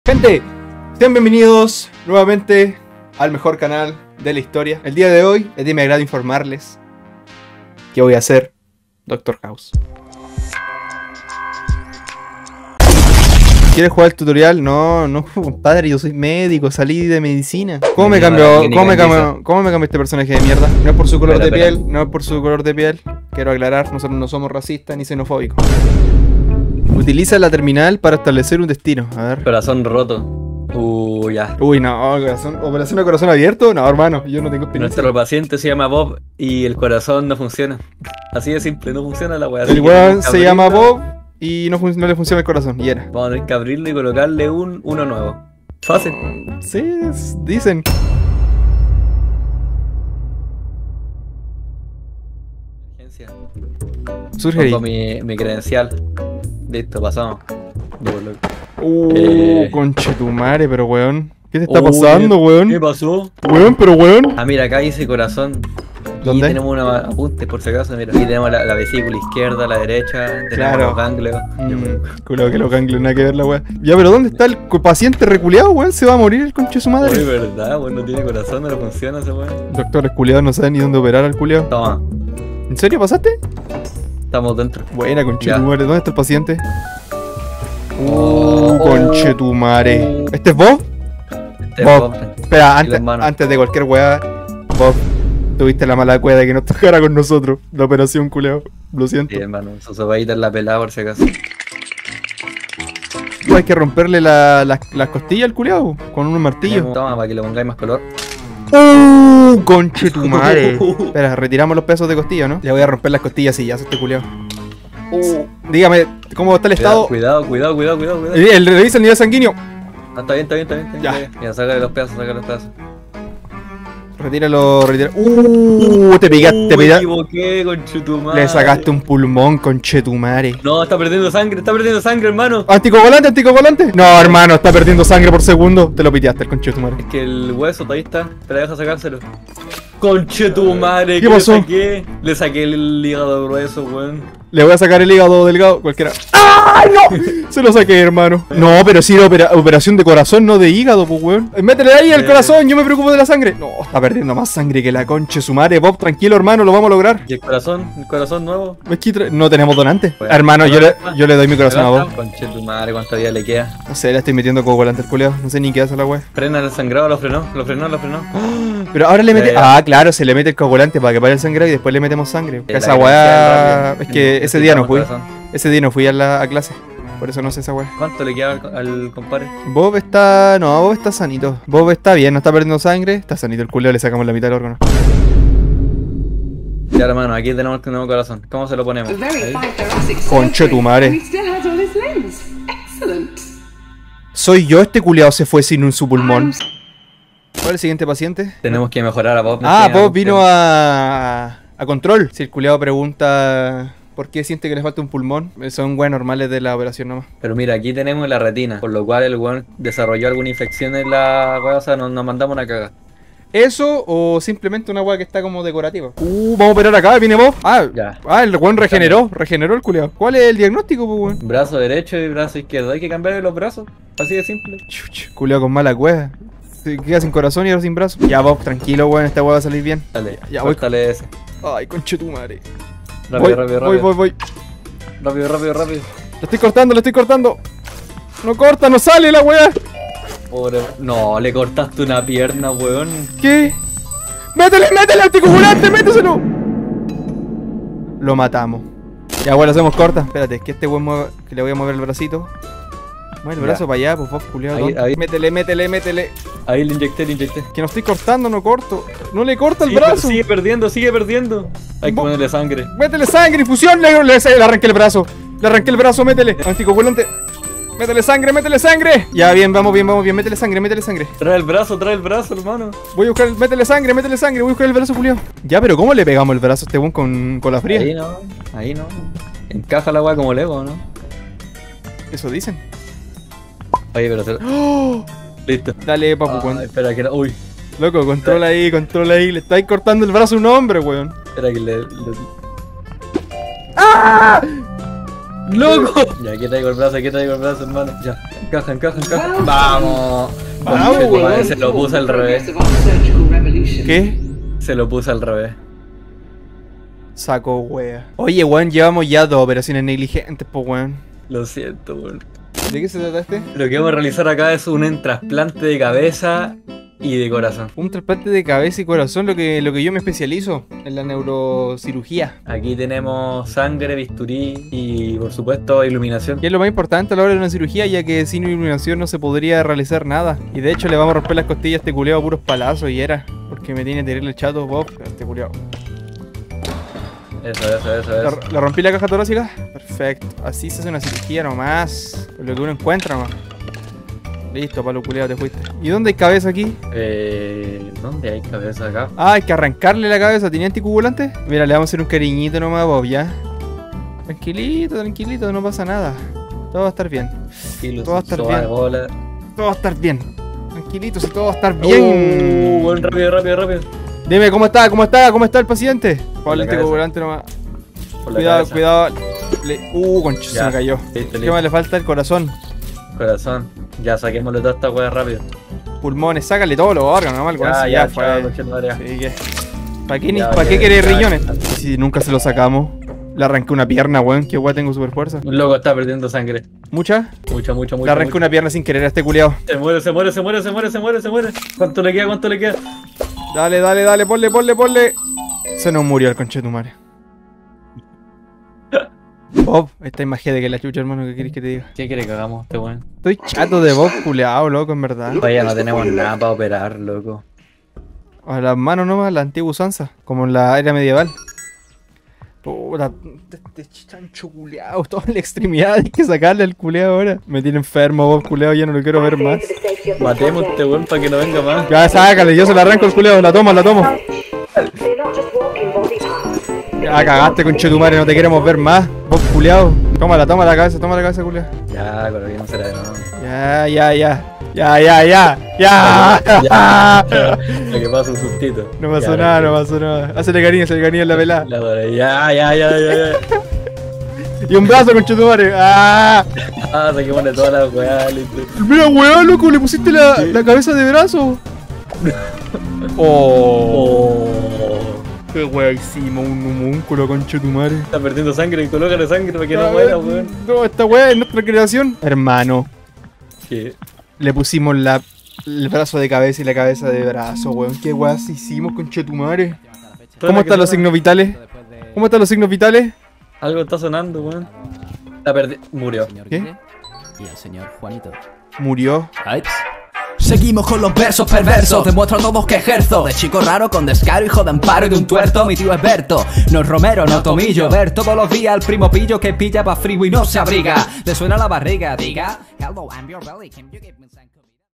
EL DÍA QUE FUI CIRUJANO
Bob perdón por todo...

00:00 Introducción
00:48 Primer paciente
03:17 Segundo paciente
05:13 Tercer paciente
06:40 Locura máxima

CANAL DEL NIÑO: @Shadoku